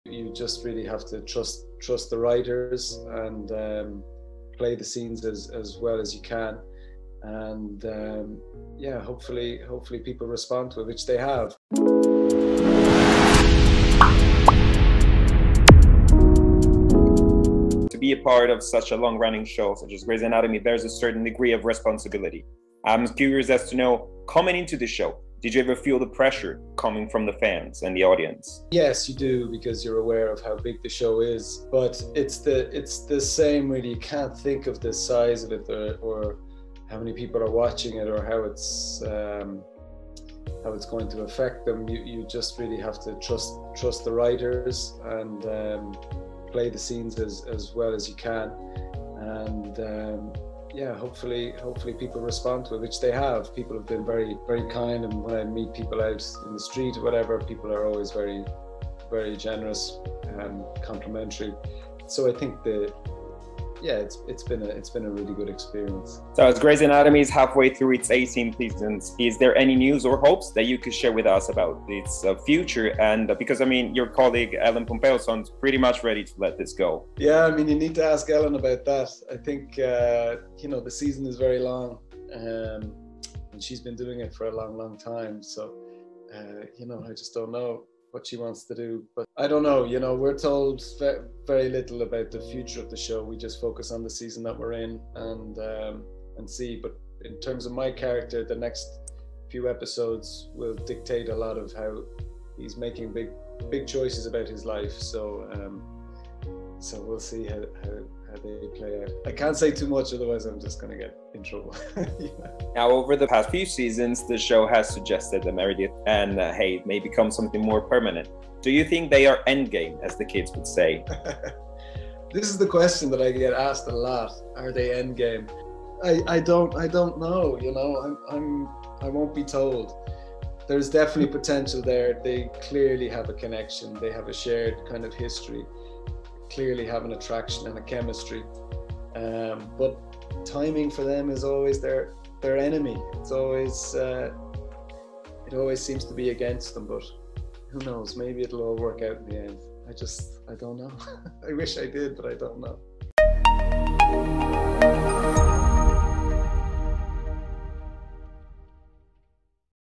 Grey's Anatomy - Tvserial.it intervista Richard Flood
Intervista a RichardFlood interprete di Cormac Hayes in GreySAnatomy